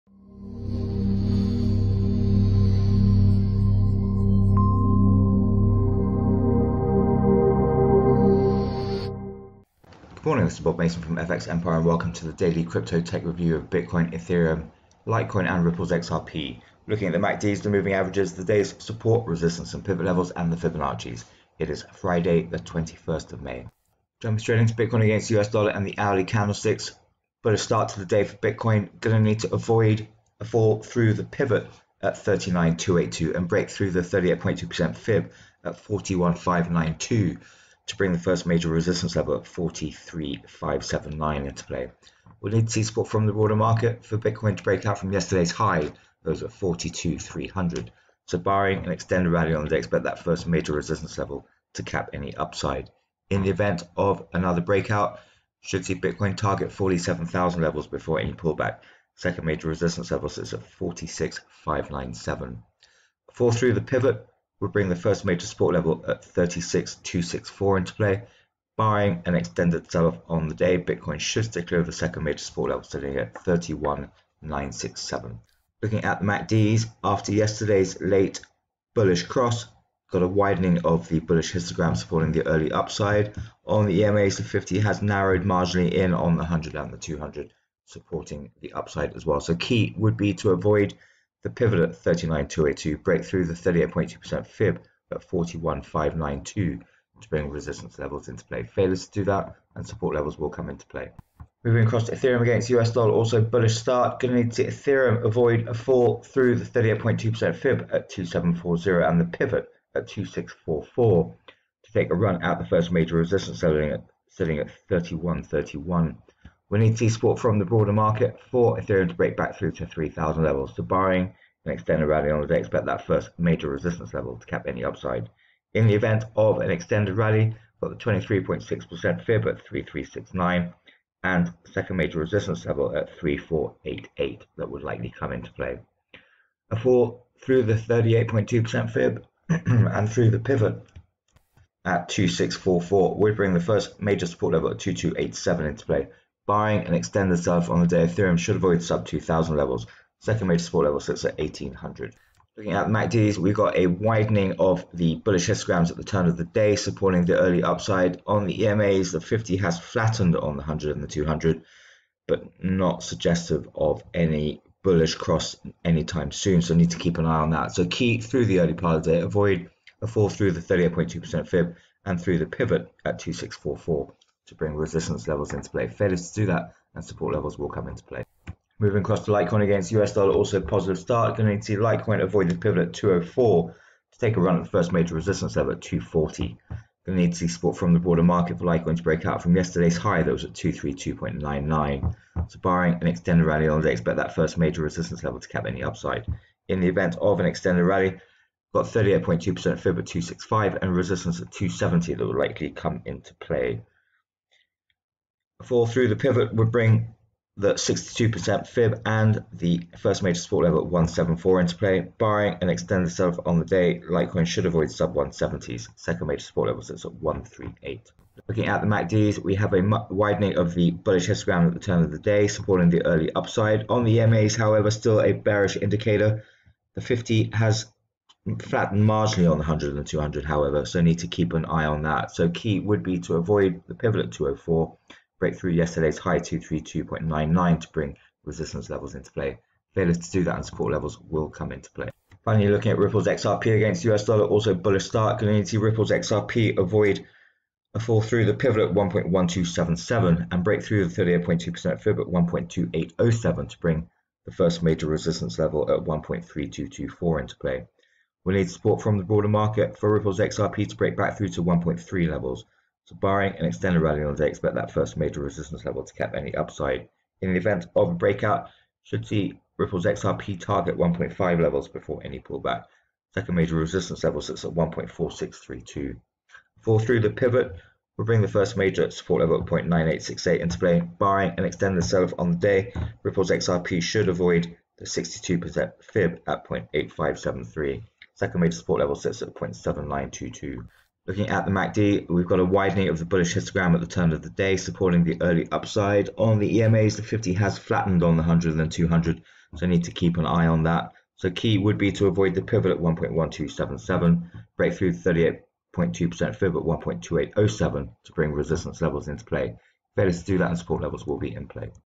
Bitcoin, Ethereum, Litecoin, and Ripple's XRP Daily Tech Analysis May 21st, 2021
It's a bullish start to the day for the majors. Steering clear of the day's pivot levels would support another day of solid gains.